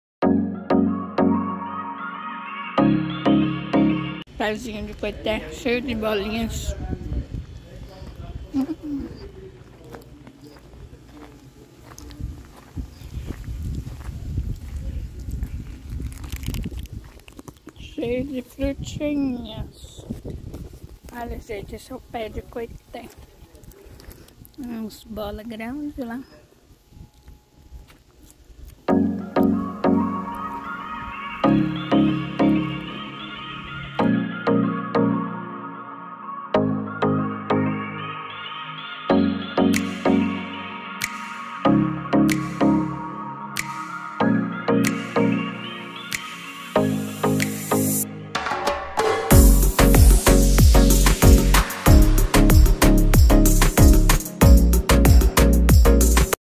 OLHA QUE MARAVILHA UM PÉ DE COITÉ COM FRUTOS! - @CANAL APRENDIZ
Vejam  que maravilha um pé  coité!
Crescentia cujete, popularmente chamada de cuia ou coité, é uma planta medicinal já conhecida de forma popular com ação anti-hipertensiva contra diabetes, infecção urinária, com ação abortiva e antimicrobiana cutânea.